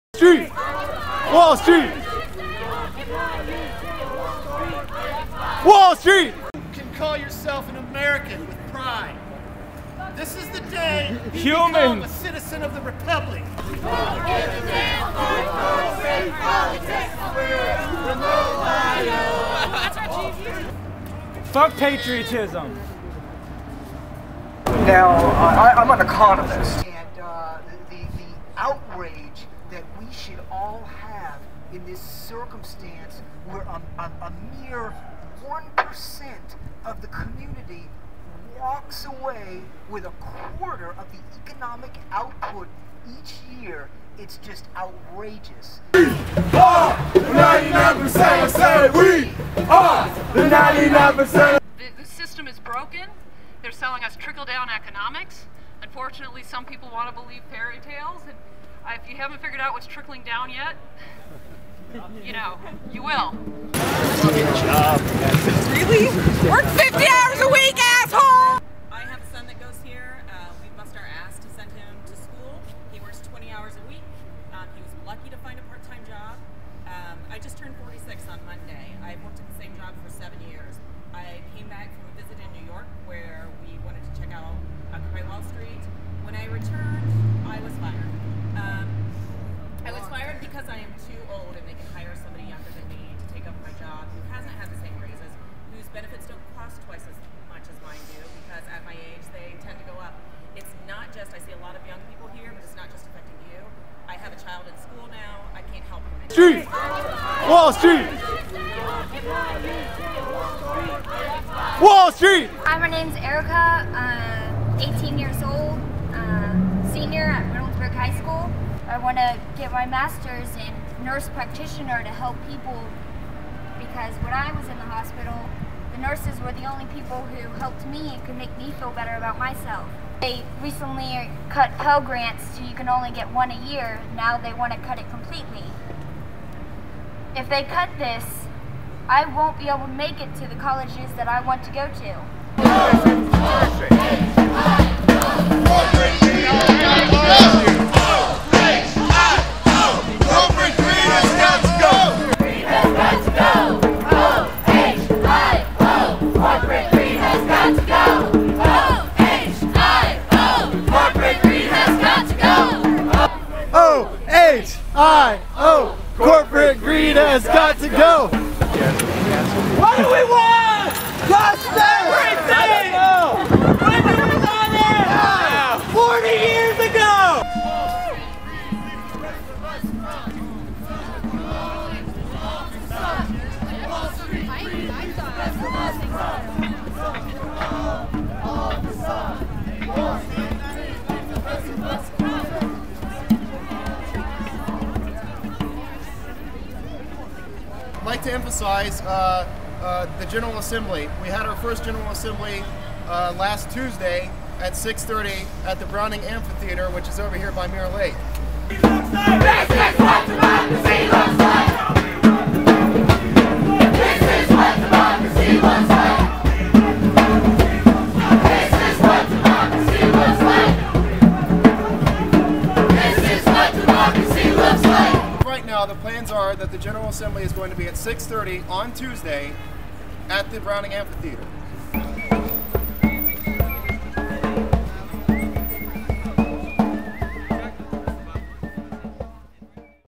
Occupy Columbus Solidarity March - October 1st
This is video from the Occupy Columbus Solidarity March that happened on October 1st 2011 at the Ohio State University and on High Street.